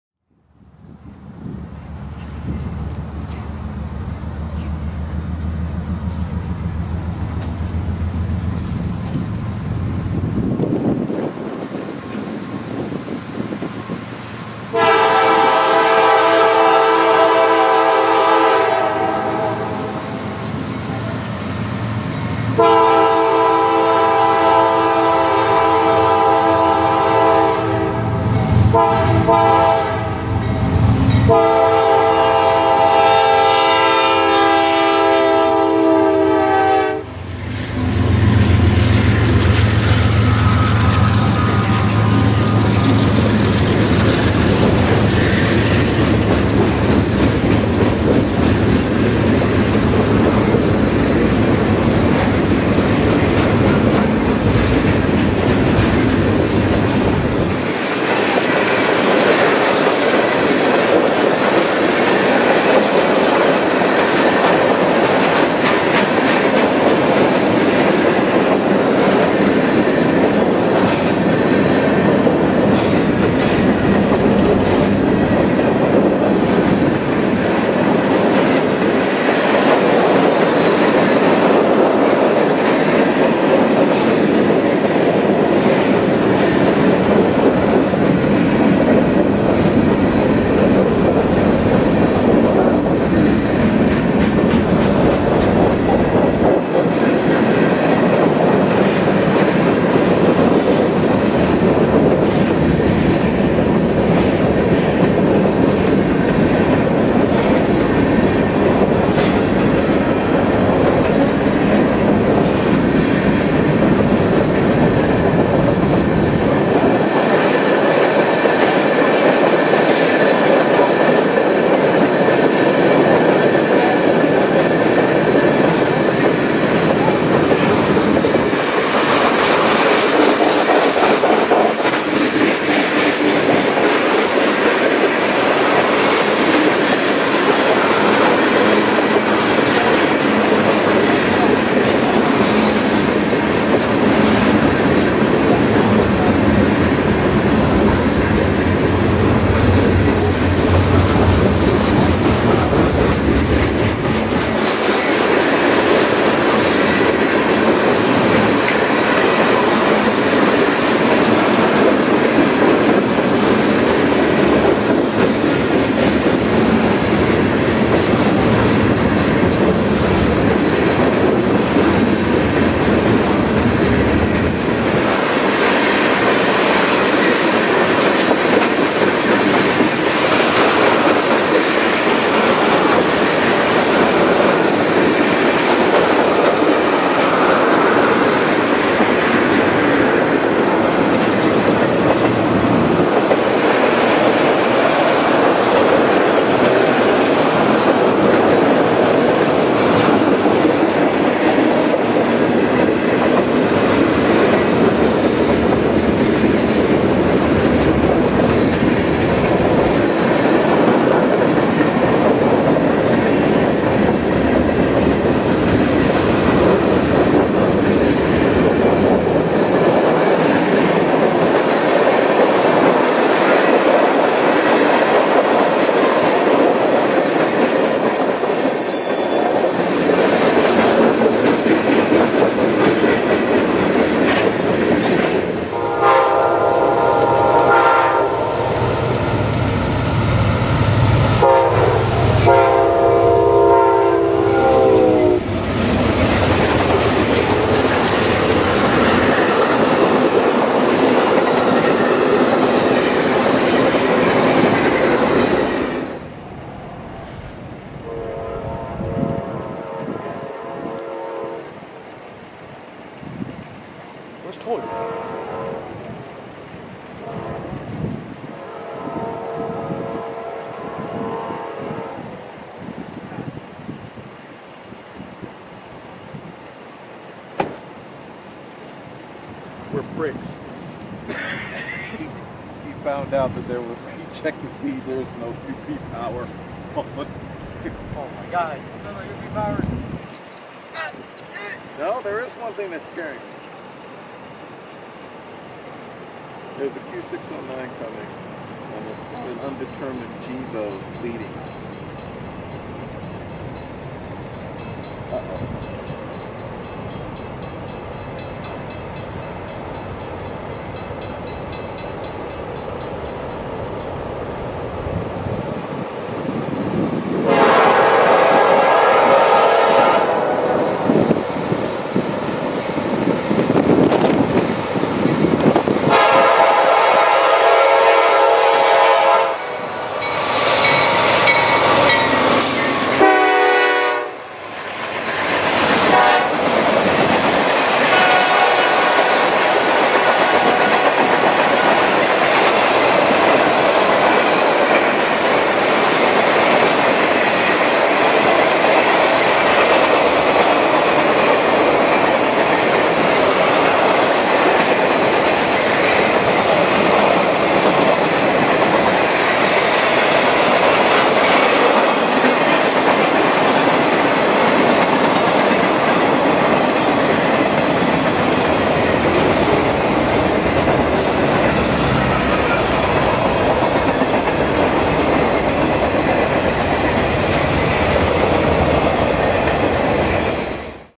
JAX BLITZ Q602 P098 S473
Select 1080p for the best viewing experience.

CSX Q602 in Callahan, FL

New Orleans, LA - Waycross, GA

CSXT #9040 C40-9W
CSXT #5359 ES40DC
CSXT #777  ES44AH
CSXT #1169 MP15AC

Amtrak P098 in Callahan, FL

Miami, FL - New York, NY

Amtrak#57    P42DC

CSX S457

Busch, FL - Waycross, GA

CSXT #7827 C40-8W
CSXT #7826 C40-8W